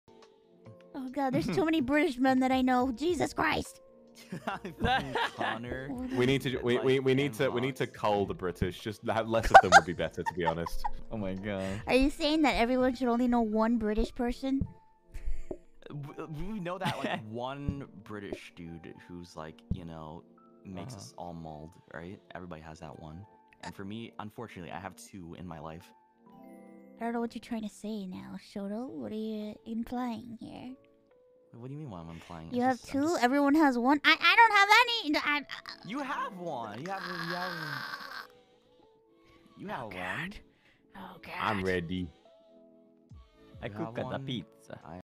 Mousey Defends Her Friendship With British Connor On Stream | IronMouse
Check out the IronMouse from her socials below.